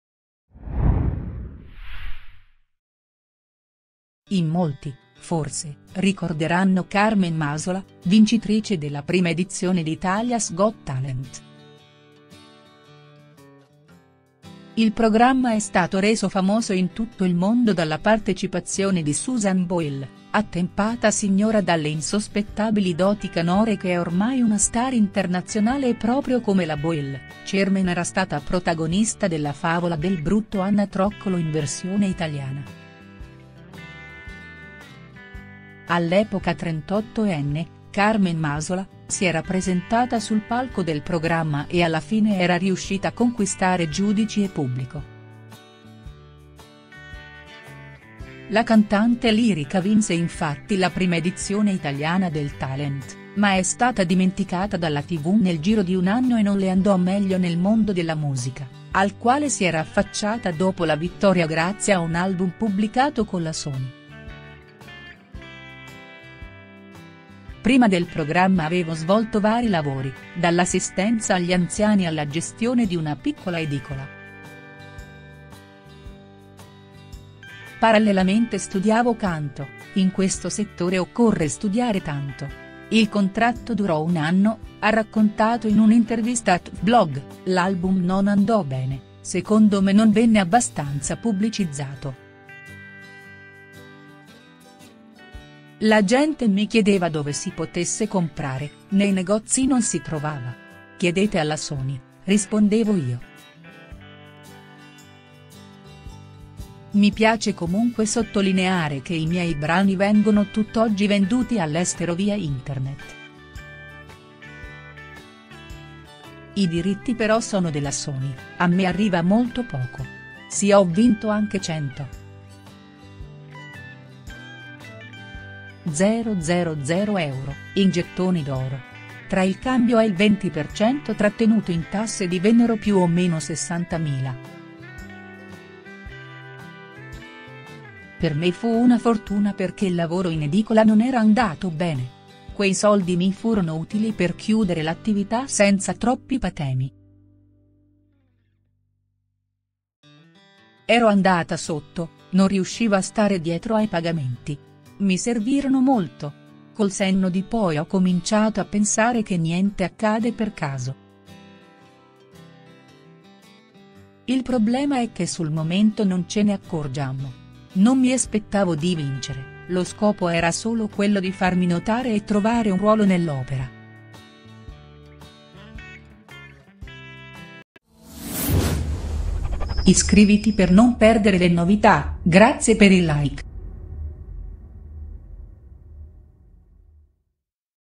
Carmen Masola, che fine ha fatto la vincitrice di Italia’s got talent
In molti, forse, ricorderanno Carmen Masola, vincitrice della prima edizione di Italia's got talent. Il programma è stato reso famoso in tutto il mondo dalla partecipazione di Susan Boyle, attempata signora dalle insospettabili doti canore che è ormai una star internazionale e proprio come la Boyle, Cermen era stata protagonista della favola del brutto anatroccolo in versione italiana. All'epoca 38enne, Carmen Masola, si era presentata sul palco del programma e alla fine era riuscita a conquistare giudici e pubblico. La cantante lirica vinse infatti la prima edizione italiana del talent, ma è stata dimenticata dalla tv nel giro di un anno e non le andò meglio nel mondo della musica, al quale si era affacciata dopo la vittoria grazie a un album pubblicato con la Sony.

“Prima del programma avevo svolto vari lavori, dall’assistenza agli anziani alla gestione di una piccola edicola. Parallelamente studiavo canto, in questo settore occorre studiare tanto. Il contratto durò un anno - ha raccontato in un'intervista a TvBlog - L'album non andò bene, secondo me non venne abbastanza pubblicizzato. La gente mi chiedeva dove si potesse comprare, nei negozi non si trovava. 'Chiedete alla Sony', rispondevo io. Mi piace comunque sottolineare che i miei brani vengono tutt'oggi venduti all'estero via internet. I diritti però sono della Sony, a me arriva molto poco".

''Sì ho vinto anche 100.000€, in gettoni d’oro. Tra il cambio e il 20% trattenuto in tasse divennero più o meno 60 mila. Per me fu una fortuna perché il lavoro in edicola non era andato bene. Quei soldi mi furono utili per chiudere l’attività senza troppi patemi. Ero andata sotto, non riuscivo a stare dietro ai pagamenti. Mi servirono molto. Col senno di poi ho cominciato a pensare che niente accade per caso. Il problema è che sul momento non ce ne accorgiamo. Non mi aspettavo di vincere, lo scopo era solo quello di farmi notare e trovare un ruolo nell’opera''.